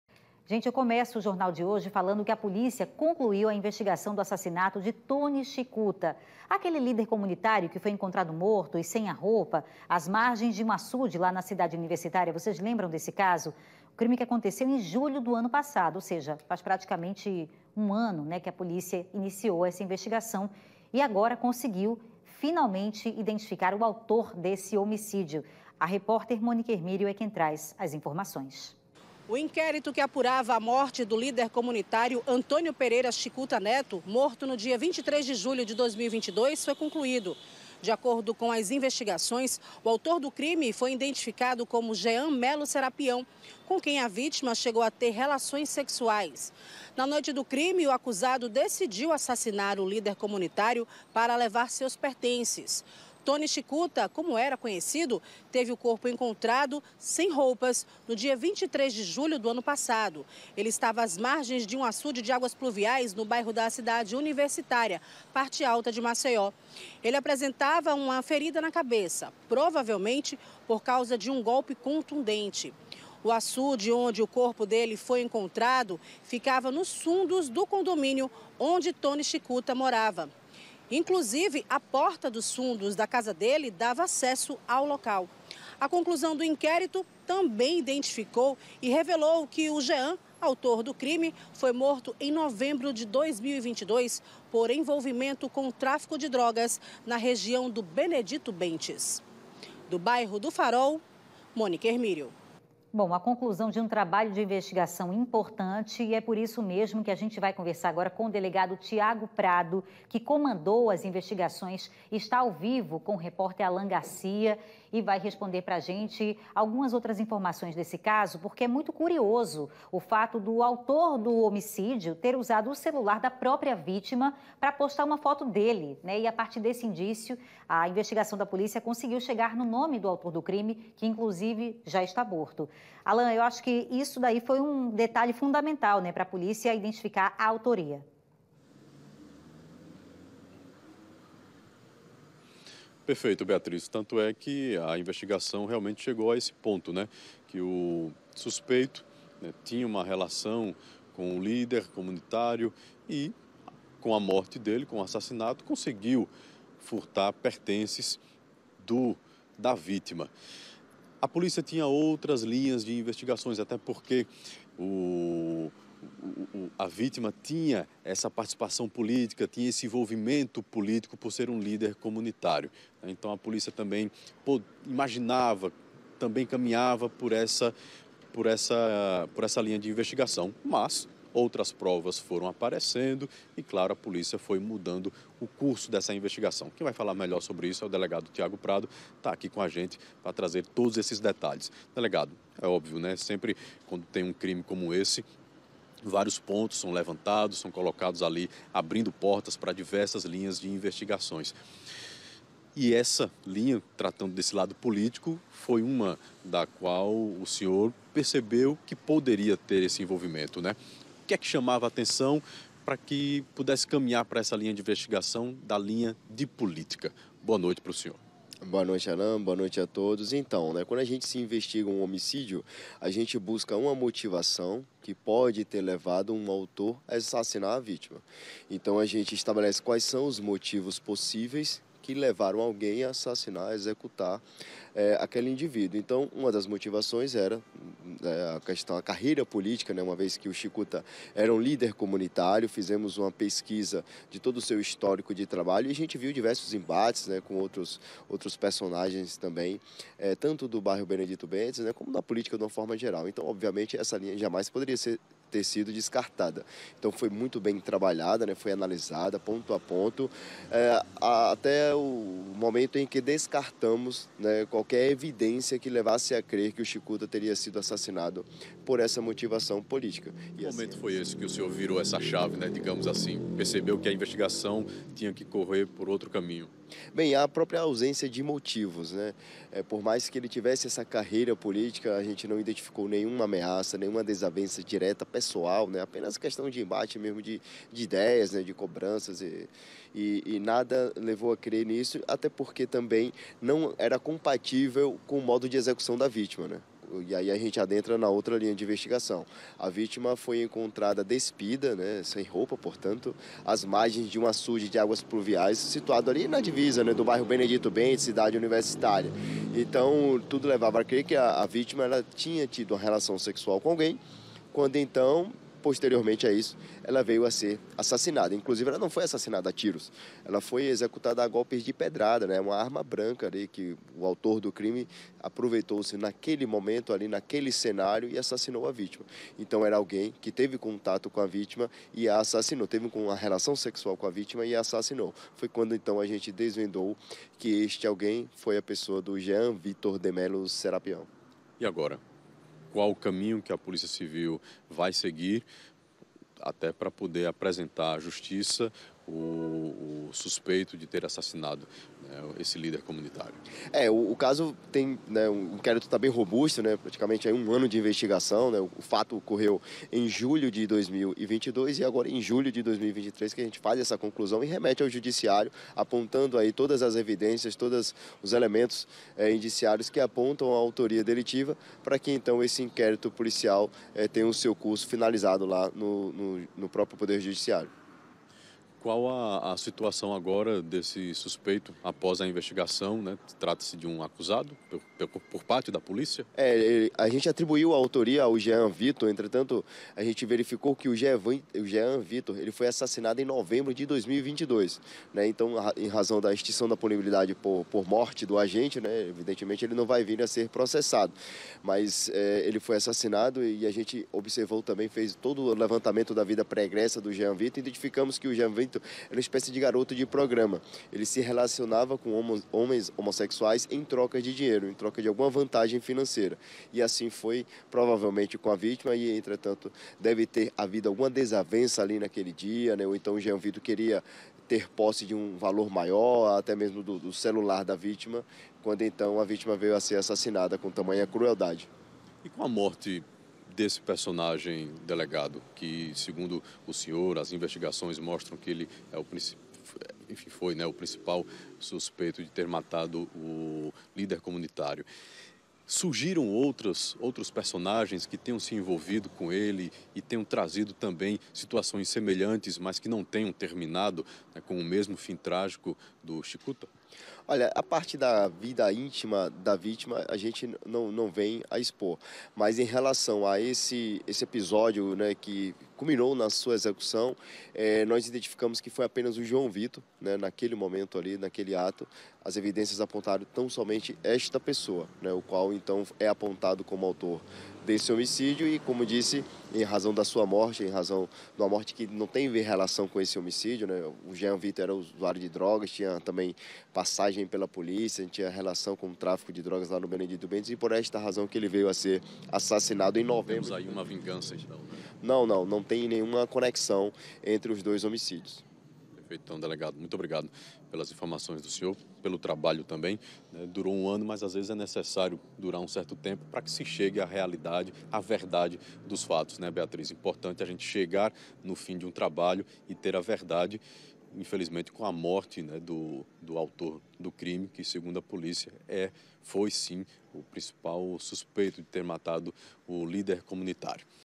Polícia conclui investigação do assassinato de líder comunitário ocorrido em 2022
Veja mais detalhes sobre o crime, na reportagem de Mônica Ermirio e Alan Garcia.